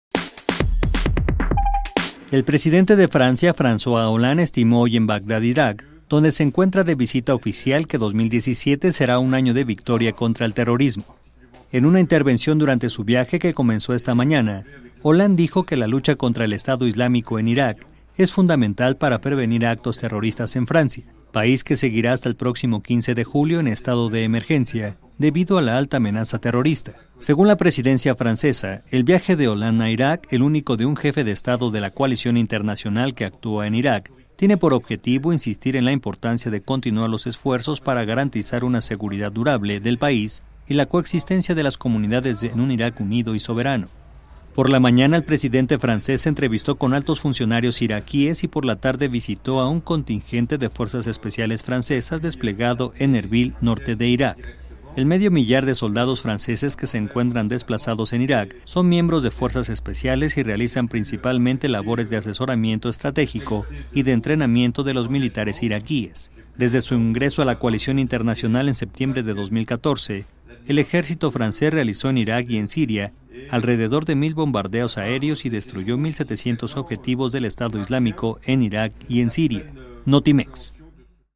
2017 será el año de victoria contra el terrorismo: Francois Hollande
El presidente de Francia, Francois Hollande, estimó en Bagdad, Irak, donde se encuentra de visita oficial, que 2017 será “un año de victoria contra el terrorismo”.